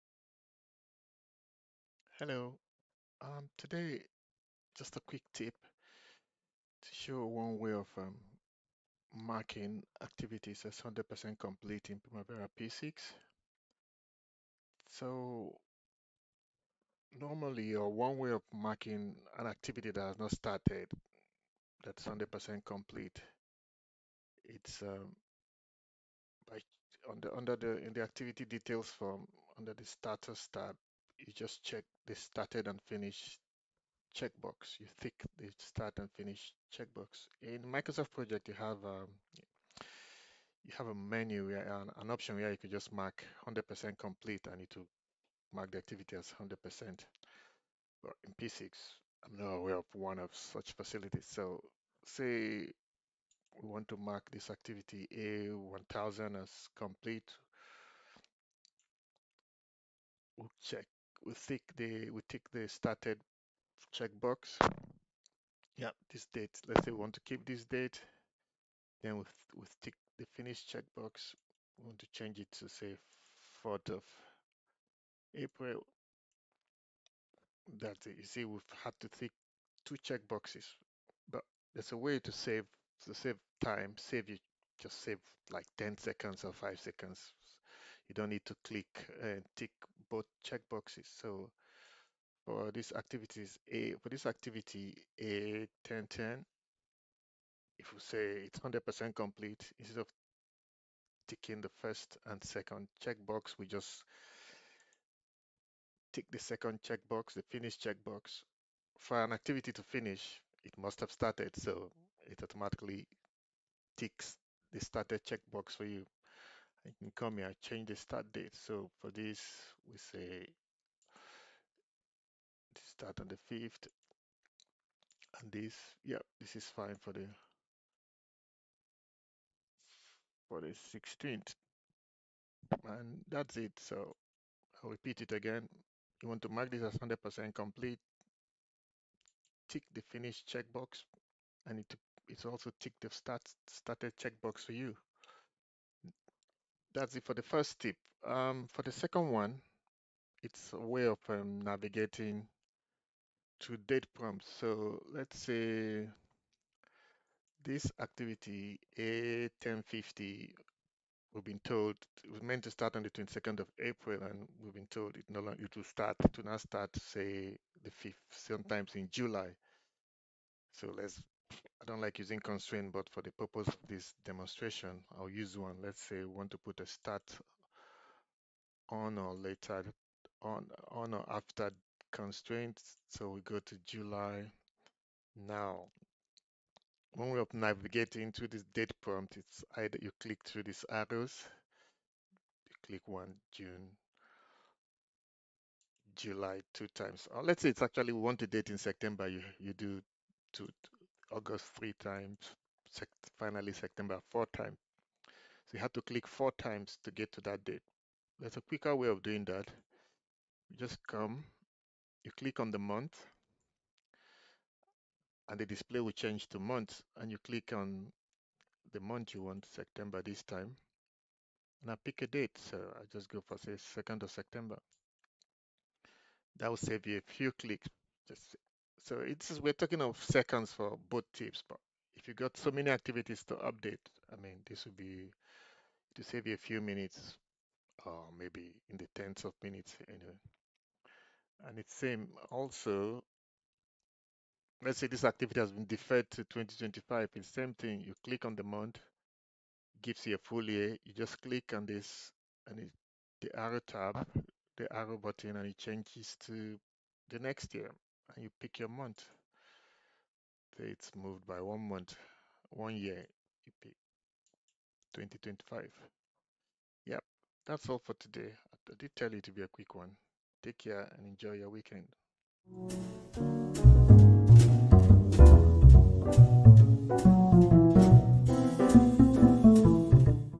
Save time when assigning Actual Dates in Primavera P6
2 shortcuts in Primavera P6 to save you time when assigning actual dates. 1st tip shows how to mark a Not Started activity as 100% complete in 1 instead of 2 clicks. And the 2nd tip shows how to quickly navigate through date prompts in P6.

Chapters in this video are:
0:00 100% Complete Shortcut
2:28 How to quickly navigate date prompts